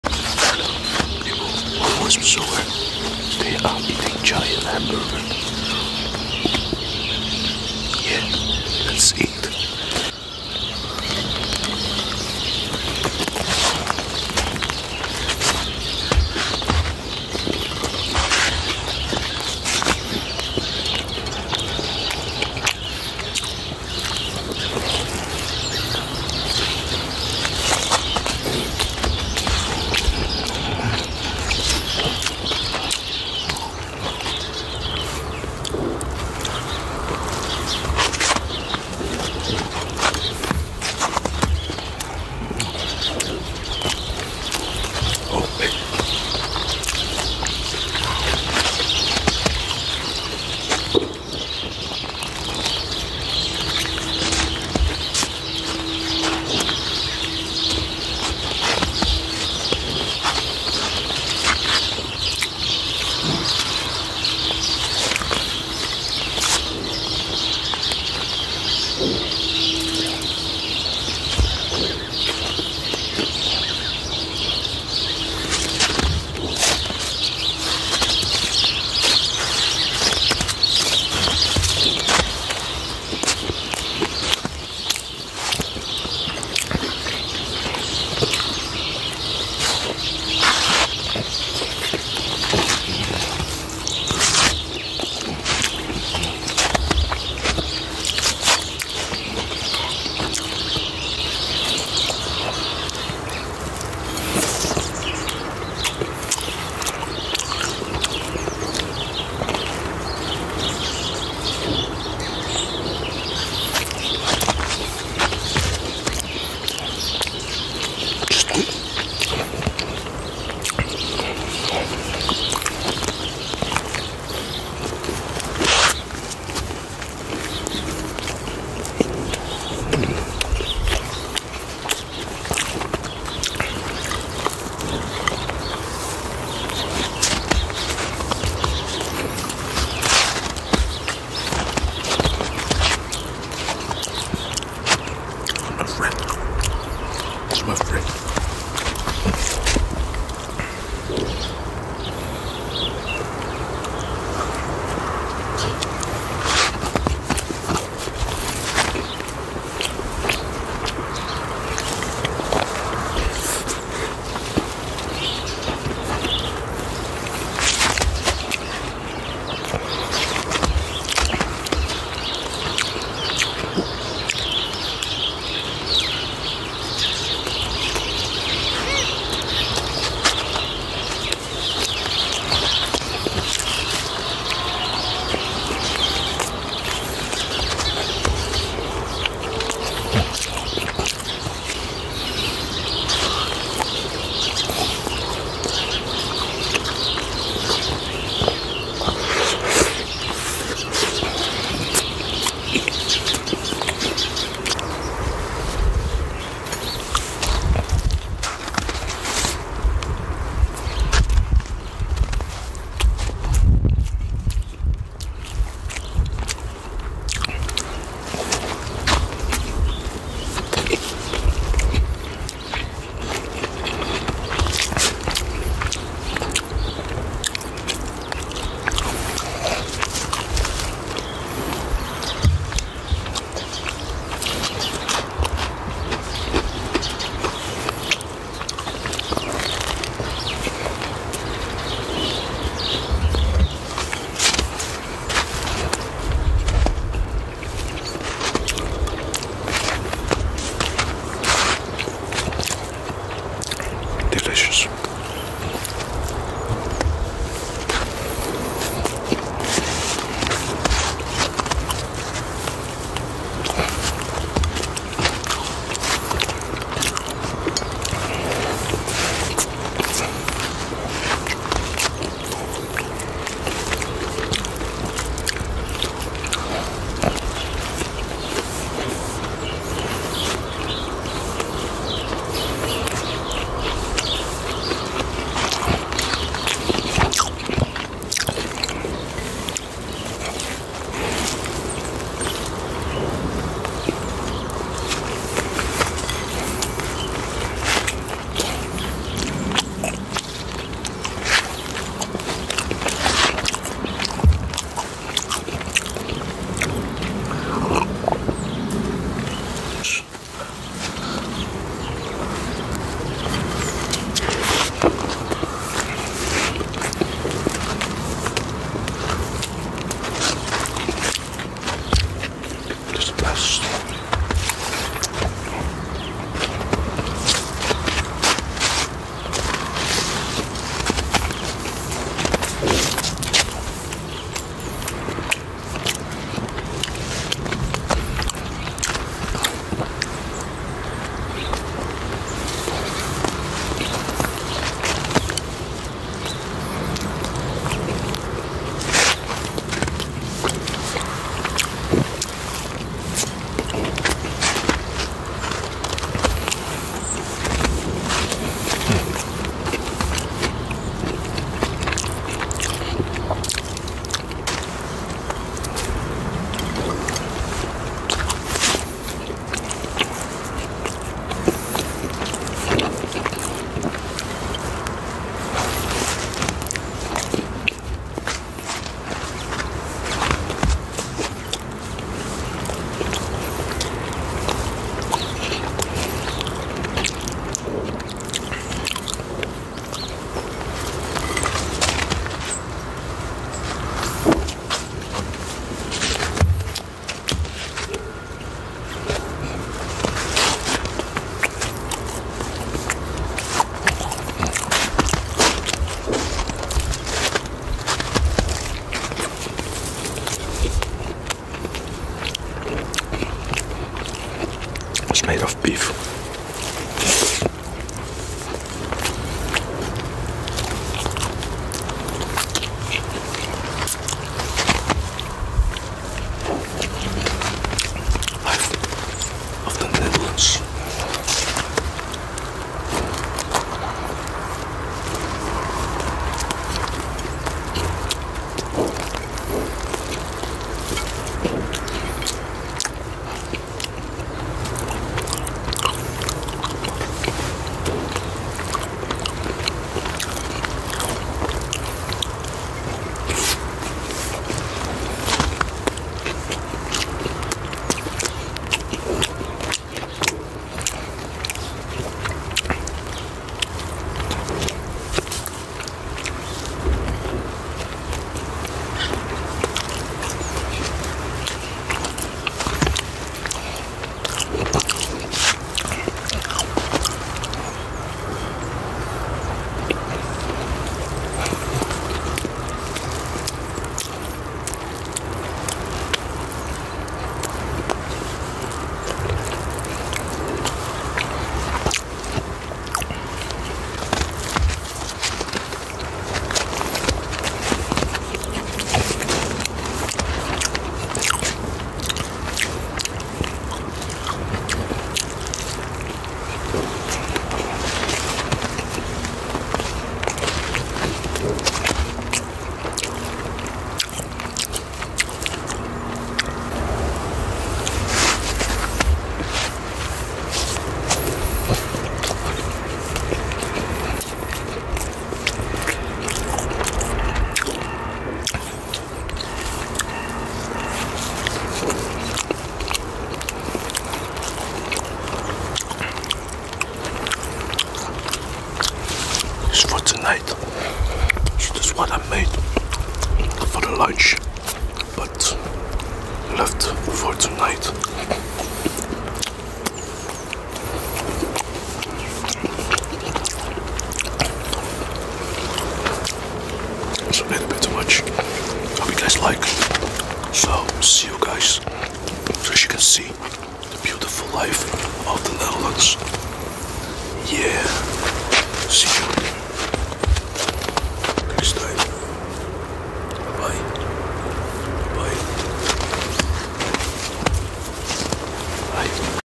ASMR GIANT HAMBURGER - Eating Sounds
In this ASMR video I'm eating a giant hamburger to produce eating sounds.